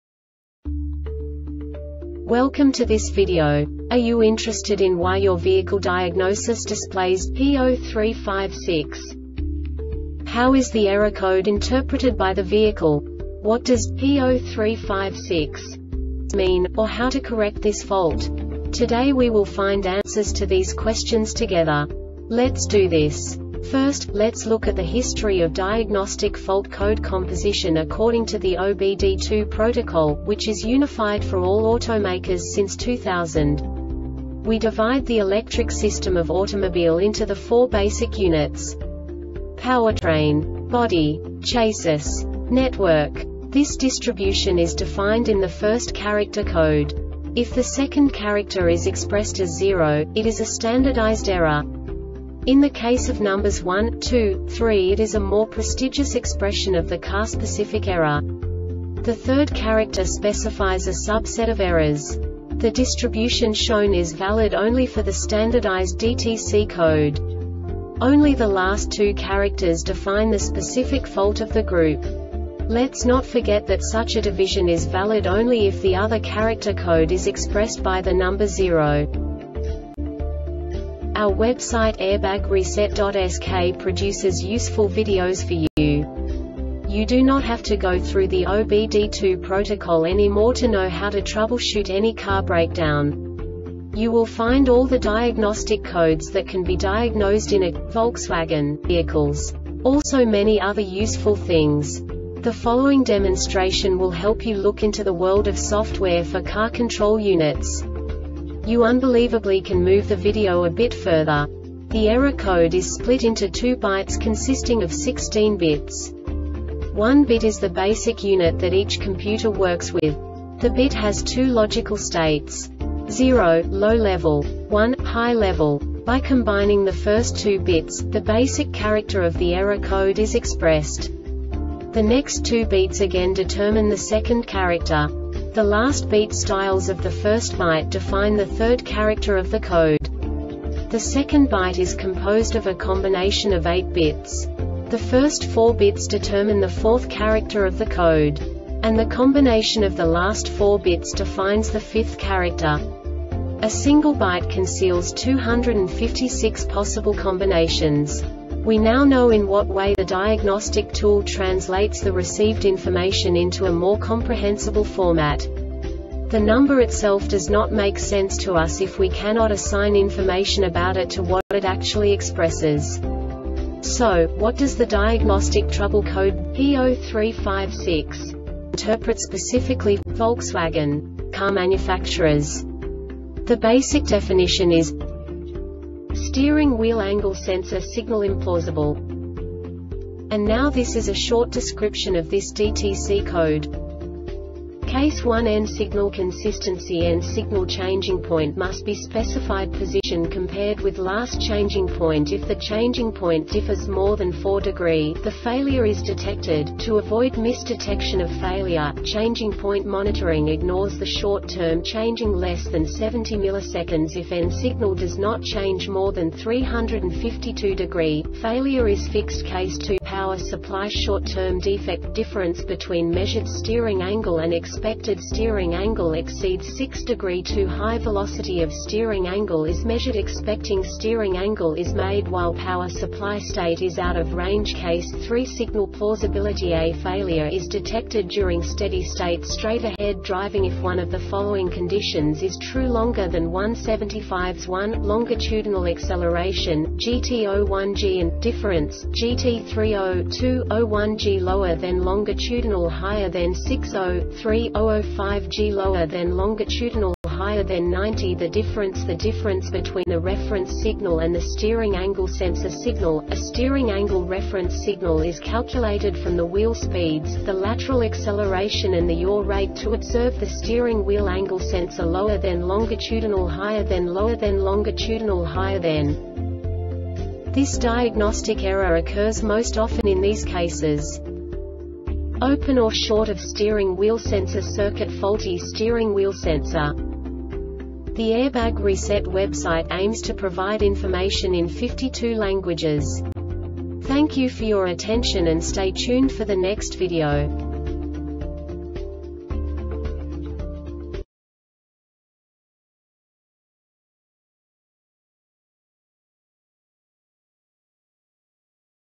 DTC Volkswagen P0356 Short Explanation
Contents:
0:21 Basic DTC analysis according to OBD2 protocol standard.
1:48 Insight into programming
2:58 A diagnostically understandable description of the Diagnostic Trouble Code
3:05 Basic error definition

Make:
Volkswagen
Code:
P0356
Definition:
Steering Wheel Angle Sensor Signal Implausible
Description:
Case 1 N-Signal Consistency N signal changing point must be specified position compared with last changing point.If the changing point differs more than 4degree,the failure is detected.- To avoid miss detection of failure,Changing point monitoring ignores the short term changing (less than 70ms).If N signal does not change more than 352 degree,failure is fixed.Case 2 Power-Supply-Short-Term-Defect Difference between measured steering-angle and expected-steering angle exceeds 6 degree.Too high velocity of steering-angle is measured.Expecting-steering-angle is made while power-supply state is out of range.Case 3 Signal Plausibility A failure is detected during steady-state straight ahead driving if one of the following conditions is true longer than 1.75s.1. |longitudinal acceleration|&gt;0.1g AND |difference|&gt;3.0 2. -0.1g lower thenlongitudinal higher then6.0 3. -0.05g lower thenlongitudinal higher then9.0 - The difference : the difference between the reference signal and the steering angle sensor signal.- A steering angle reference signal is calculated from the wheel speeds,the lateral acceleration and the yaw rate to observe the steering wheel angle sensor. lower then or longitudinal higher thenlower then or longitudinal higher then
Cause:
 Open or short of steering wheel sensor circuitFaulty steering wheel sensor